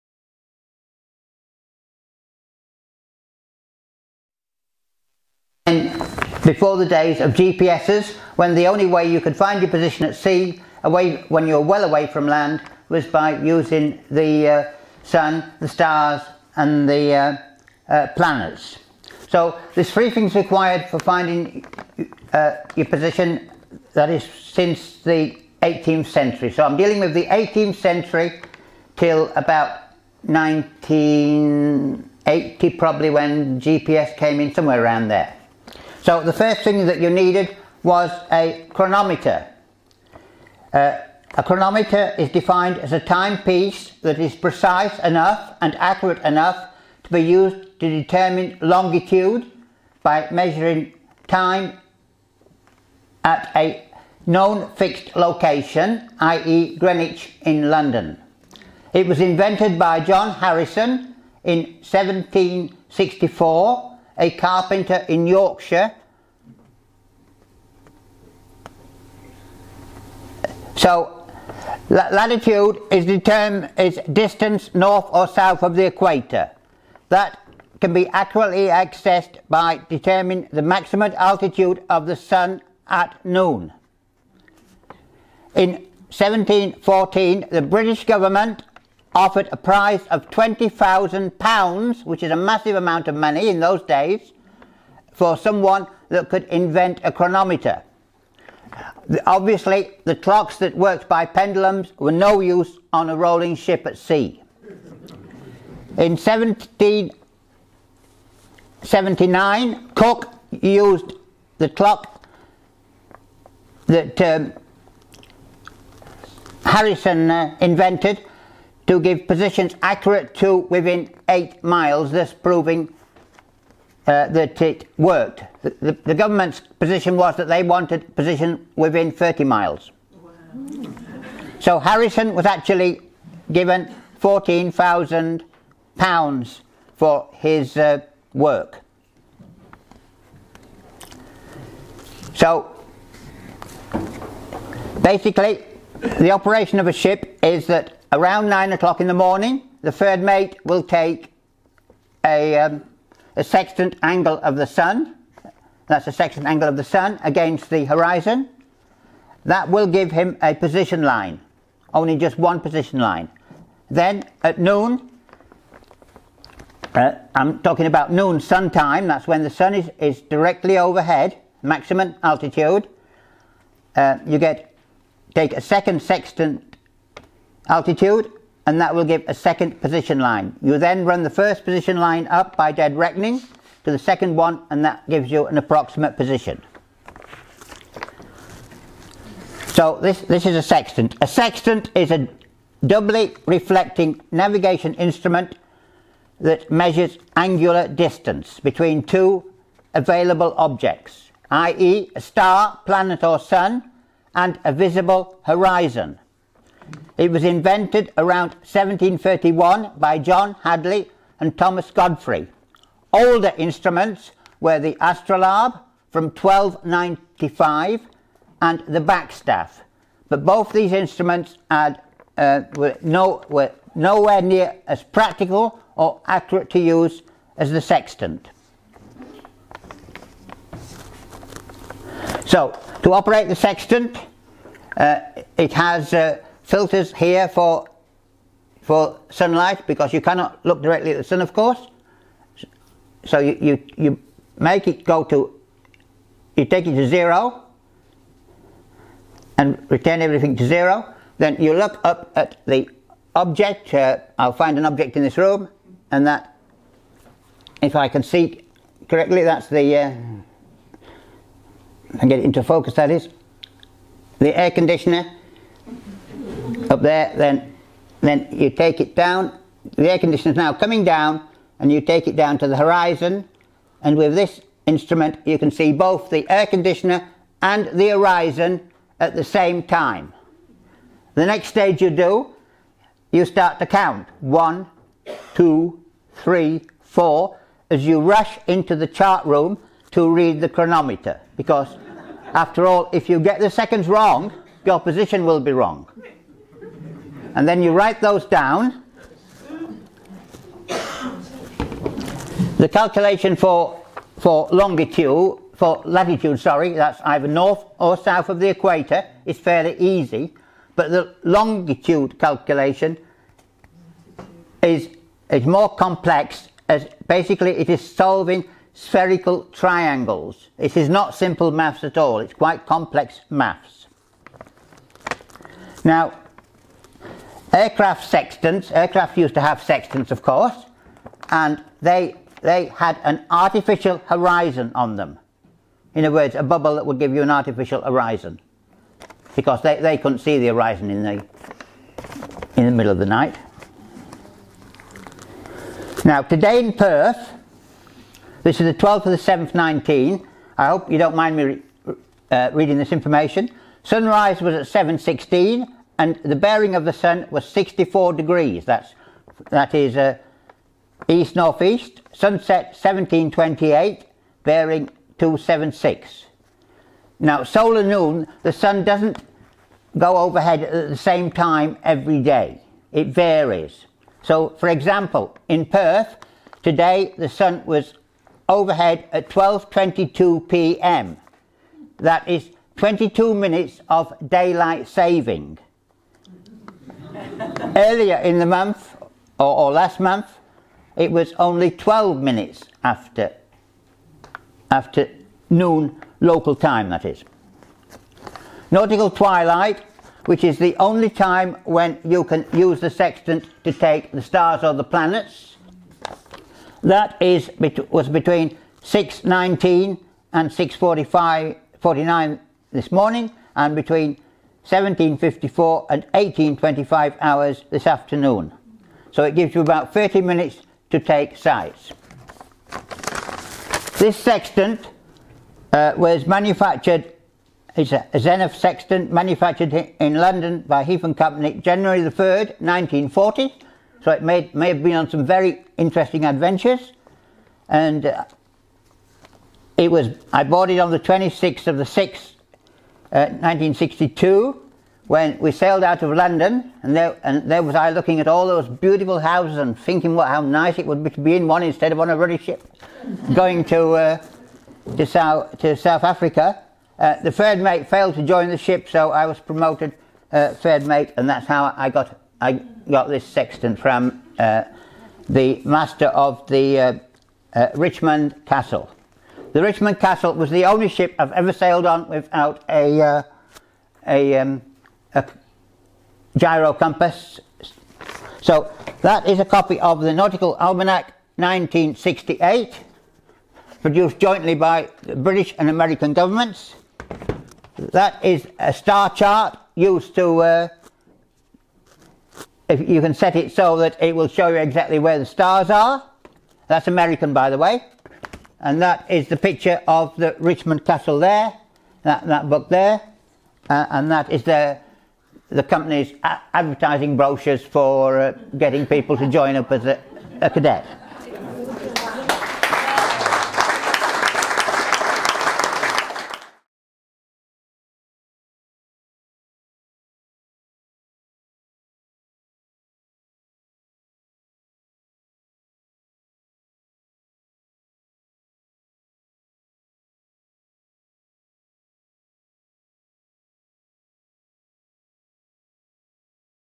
Sextant and Nautical Almanac & fixing a ship's position by the stars, by Bill Tomlinson.
The Sextant and the Nautical Almanac were used to help navigate a ship by using the sun and the stars. This is how ships found their position when they were well away from land before the advent of GPS systems in the 1980s. Bill Tomlinson explains how this was achieved and demonstrates a Sextant made in the 1940s which he used in 1962 onboard a ship: the Richmond Castle. He explains the 3 things that are needed for navigation including a chronometer. It is a true and fascinating trip back into history given at the July 2019 DRB Nats meeting to support the main speaker who spoke on the Night Sky.